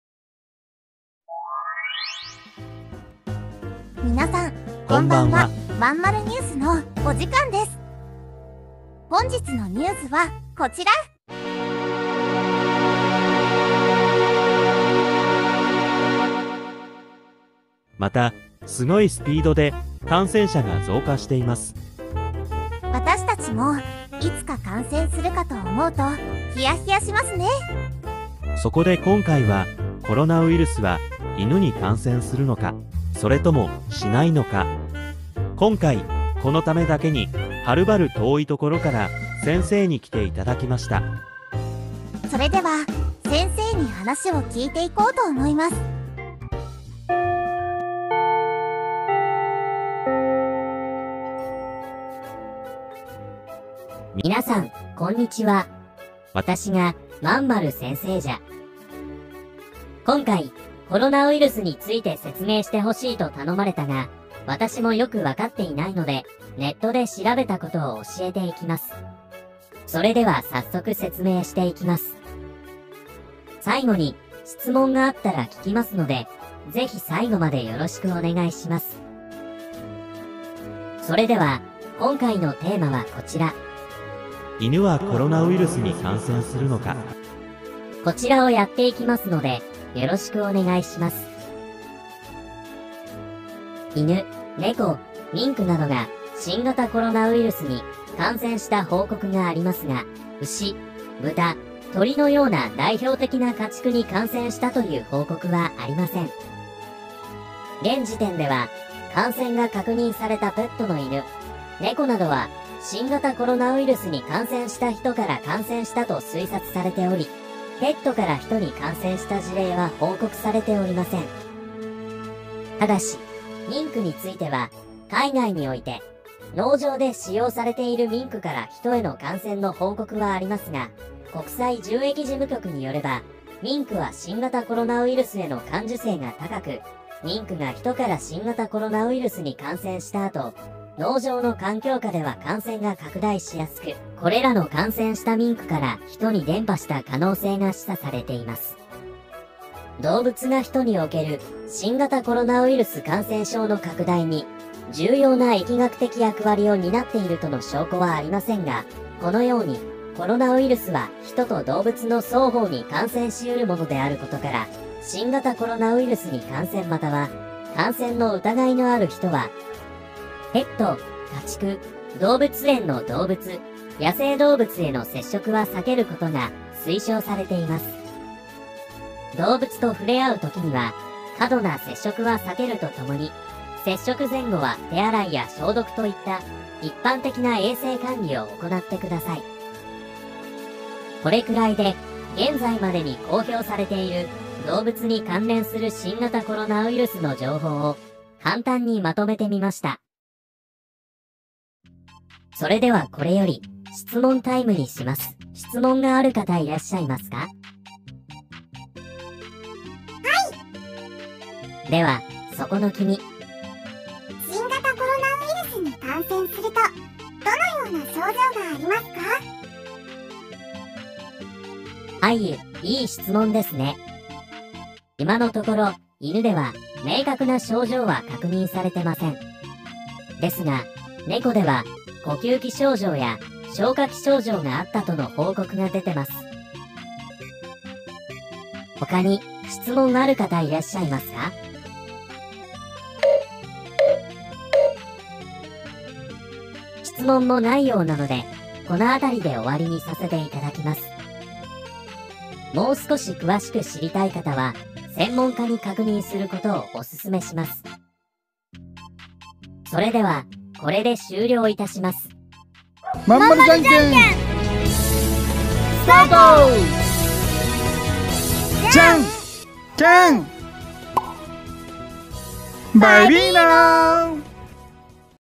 【緊急】大変な事が起きています…
再び感染者が増加しているコロナウイルス‼️
【自分のペットはコロナウイルスに感染するのか❓】
ペットを飼ってる方なら、1度は気にした事があるかと思います。
その事について、簡単にですが調べてみました🤗